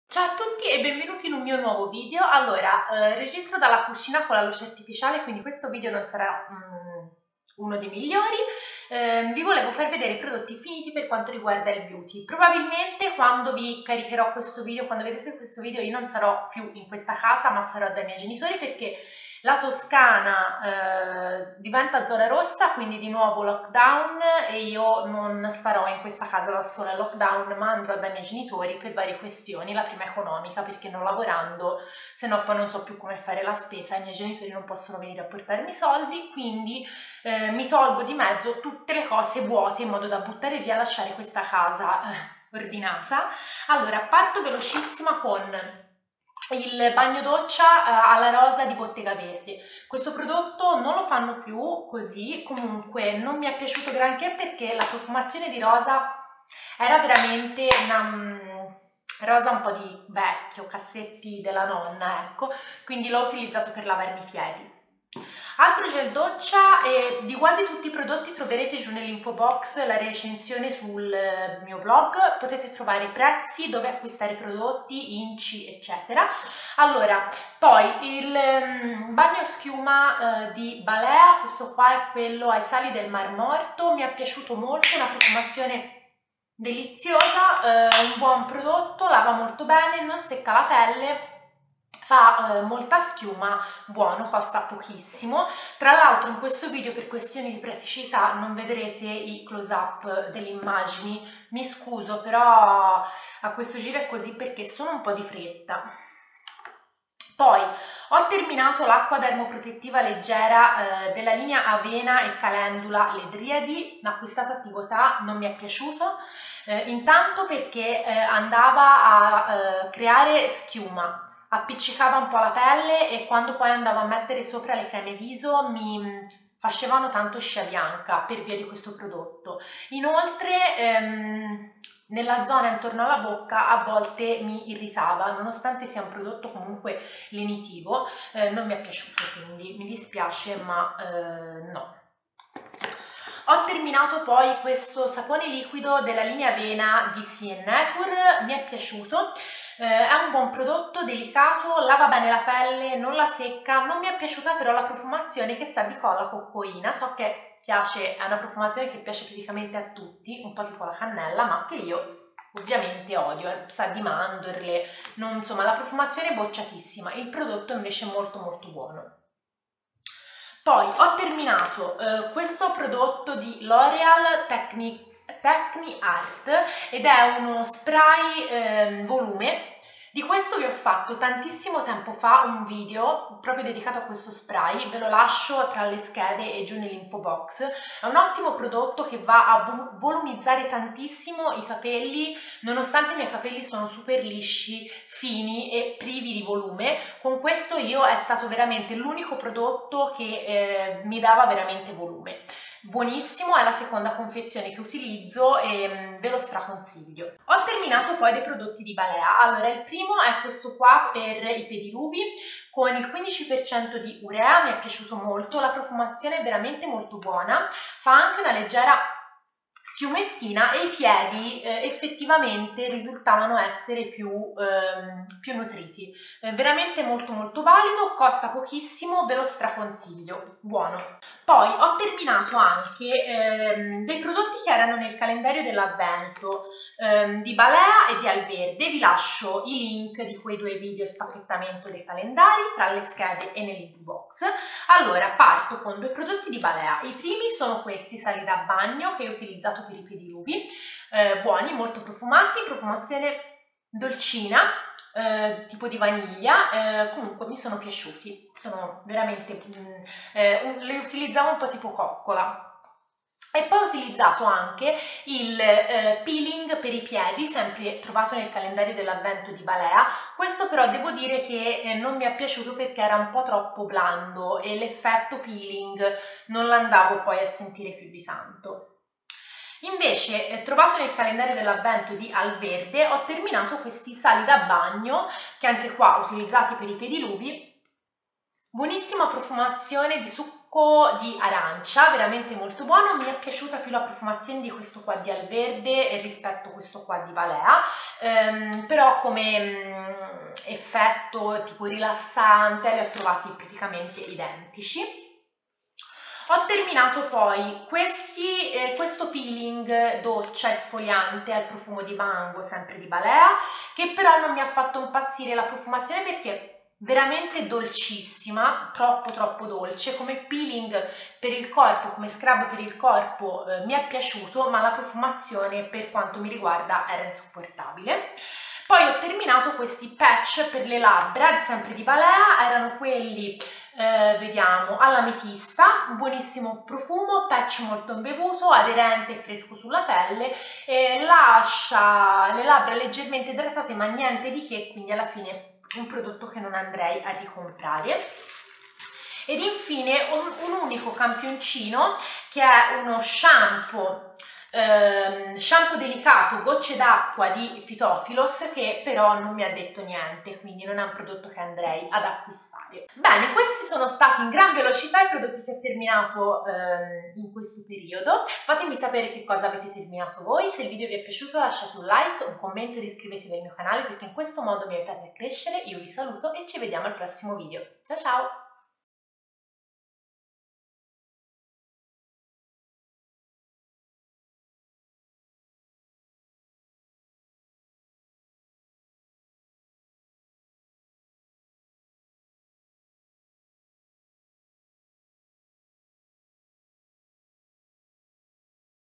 Prodotti finiti del periodo #69
Prodotti finiti del periodo

Qui sotto potete trovare i post con alcuni prodotti citati nel video nel dettaglio (inci a pallini, info sul prodotto e recensione,...):

🌷Balea sali da bagno con sale marino ed olio di cocco